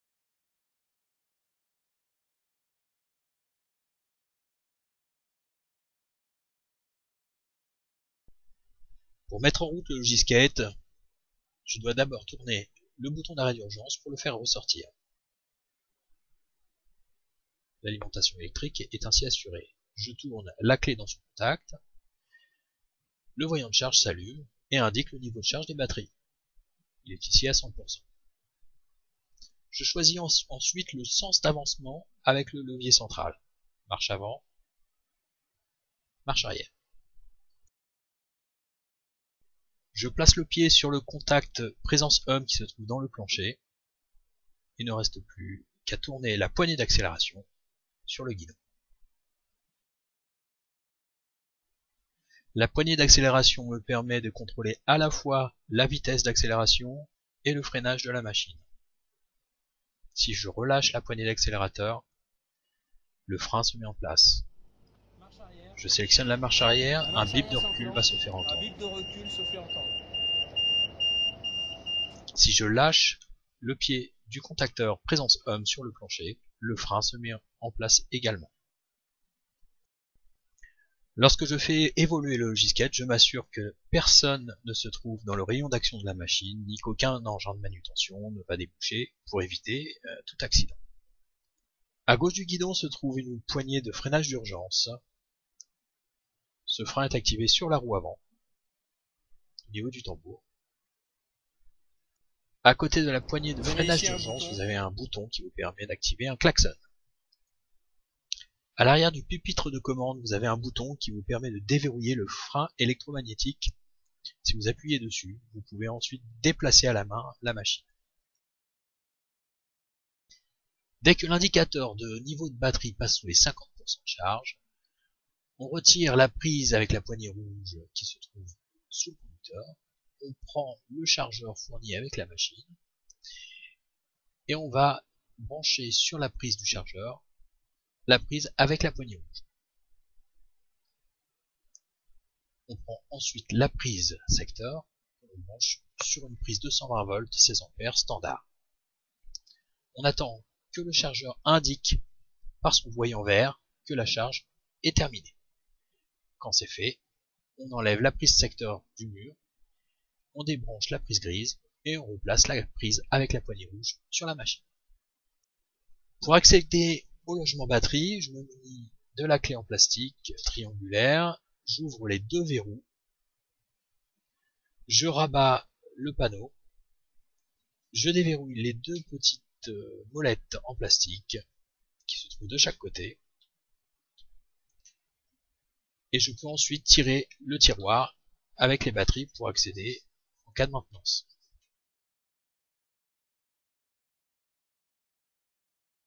Utilisation d'un LOGISKATE d' Electroman
Vidéo qui explique comment utiliser le Logiskate, tracteur/Transporteur électrique à personnel embarqué pouvant tracter jusqu'à 1000kg.
Personnalisable une multitude d'accessoires existe pour cette machine.